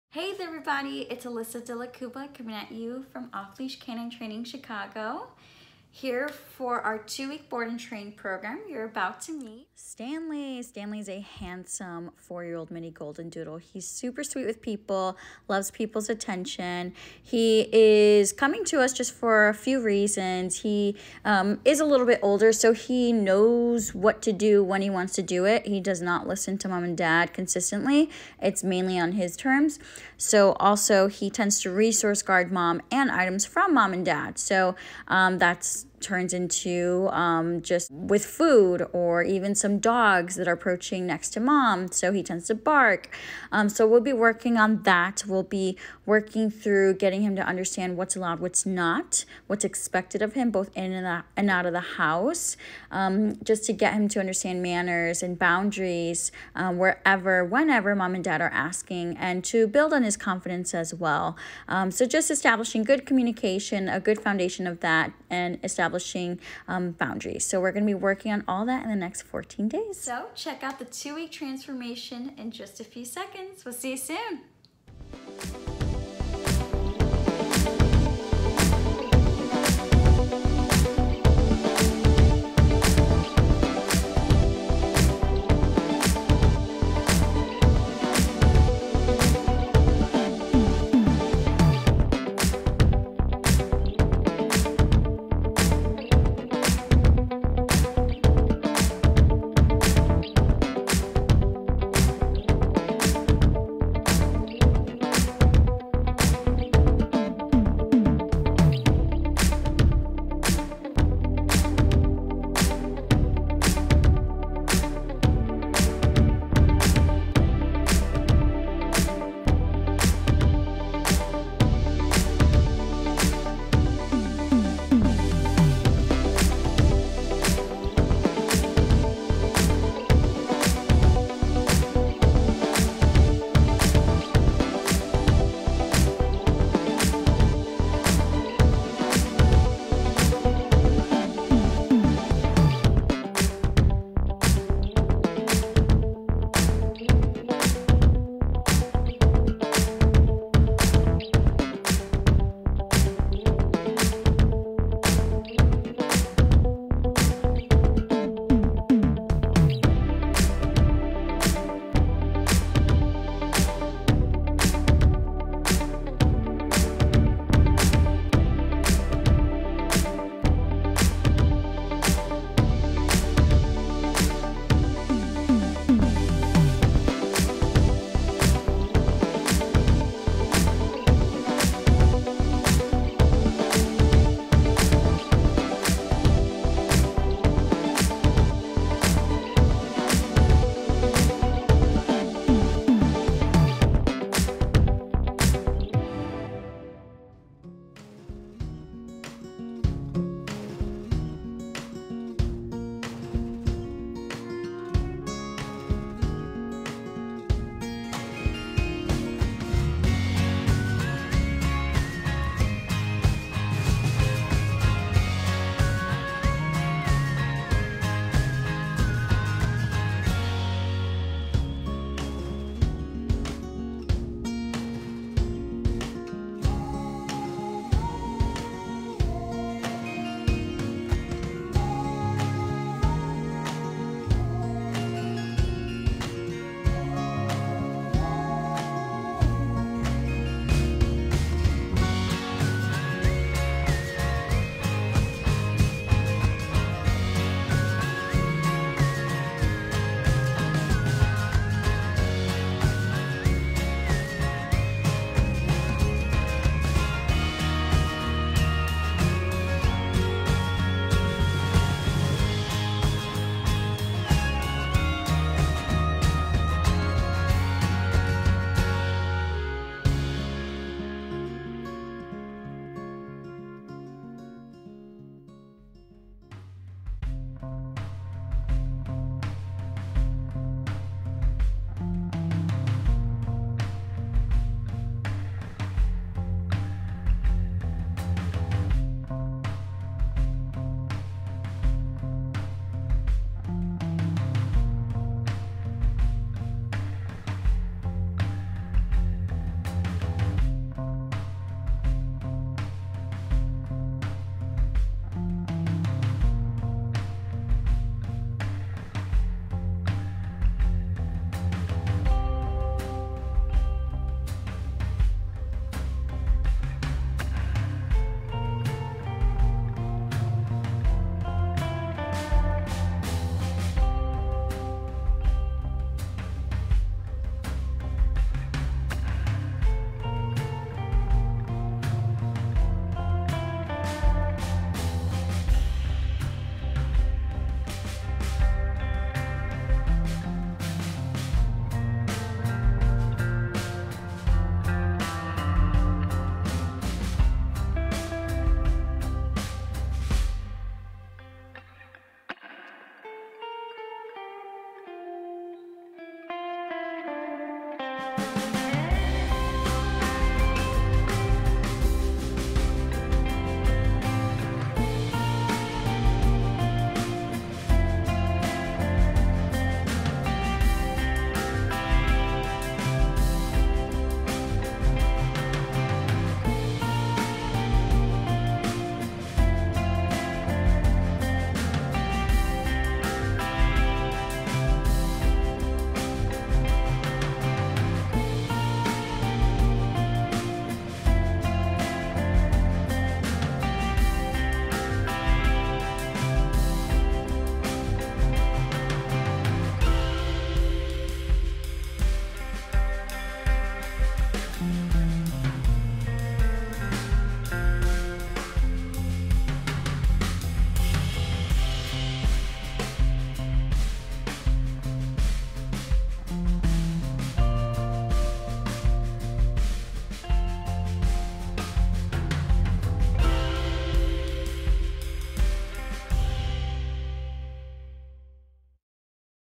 Best Dog Training in Chicago! 4 Year Old Mini Goldendoodle, Stanley!
Take a look at the progress we were able to make with Stanley in just 2 weeks!